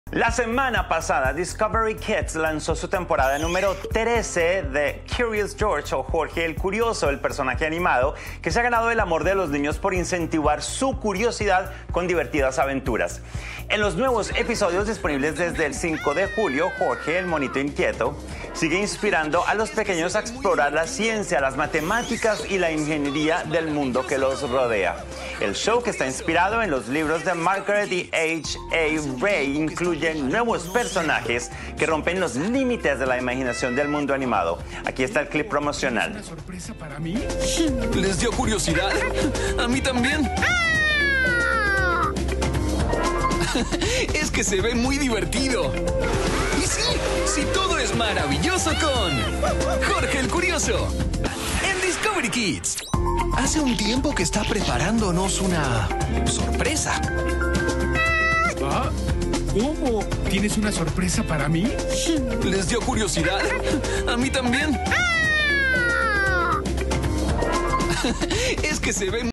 Discovery Kids estrenó la XIII temporada de “Jorge el Curioso”
La semana pasada Discovery Kids lanzó su temporada número 13 de “Jorge el Curioso”, el personaje animado que se ha ganado el amor de los niños por incentivar su curiosidad con divertidas aventuras.

En los nuevos episodios, disponibles desde el 5 de julio, Jorge, el monito inquieto, sigue inspirando a los pequeños a explorar la ciencia, las matemáticas y la ingeniería del mundo que los rodea.

El show, que está inspirado en los libros de Margret y H. A. Rey, incluye nuevos personajes que rompen los límites de la imaginación del mundo animado.